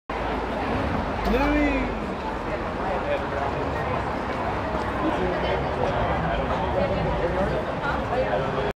Louie Vito departs the Scary Movie 5 Premiere in Hollywoo...
Louie Vito departs the Scary Movie 5 Premiere in Hollywood, 04/11/13